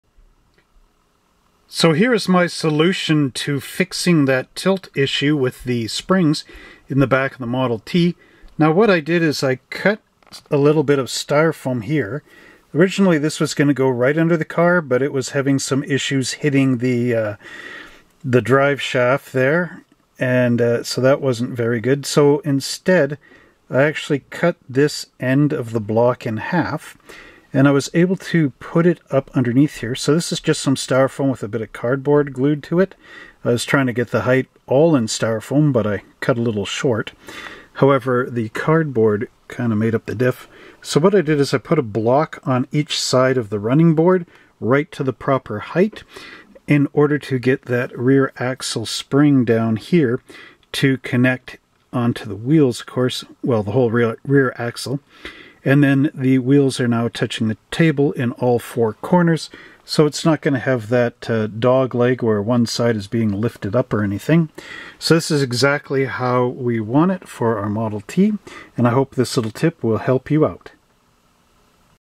A Simple Way To Get Perfect Spring Alignment On Your AMT 1925 Ford Model T Kits.
Here's how I got my AMT 1925 Ford Model T suspension perfectly aligned to avoid the dreaded 1 wheel lift, where only 3 wheels touch the table top and one is off.